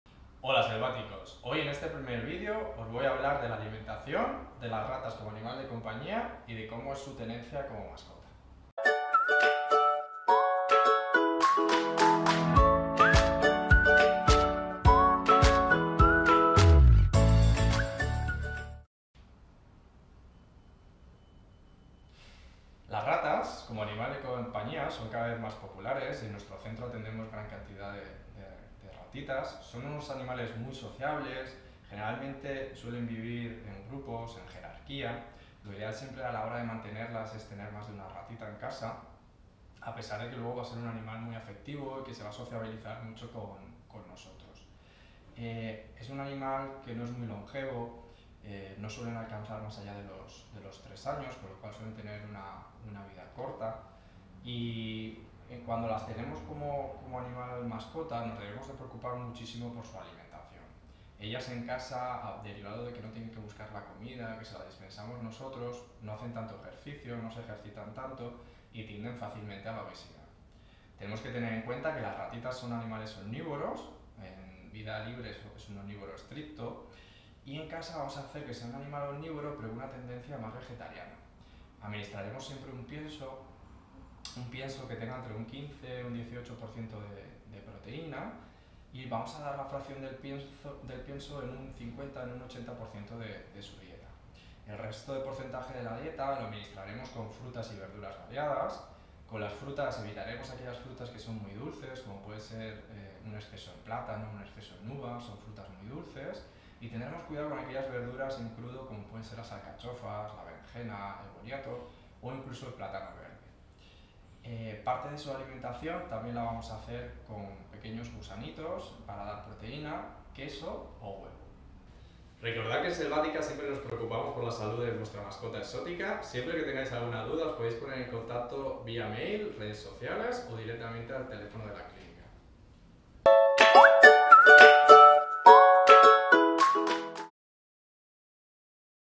Como son las ratas como mascotas y su alimentación
Hola Selväticos, en esta primera parte os explicamos como son las ratas como mascotas y los cuidados que hay que tener en cuenta en su alimentación. A diferencia de lo que la gente piensa las ratas son magnificas mascotas para los niños ya que no muerden, son inteligentes e interactúan con las personas.